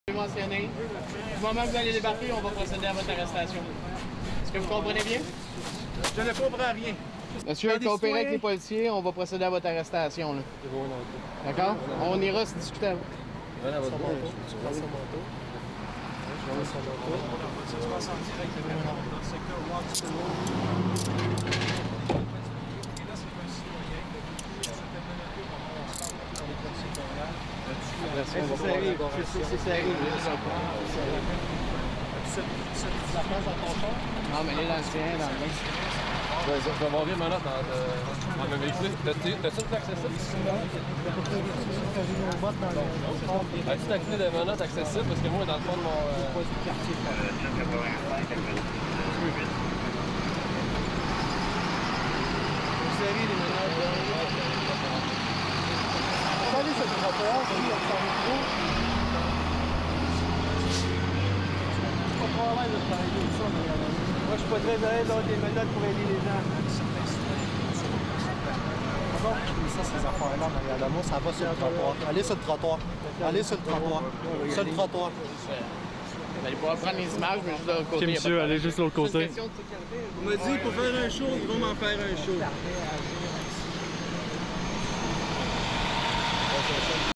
Montreal man arrested for bringing sandbags to flooded street
Alain Furlano was arrested by Montreal police for trying to deliver sandbags to a flooded street.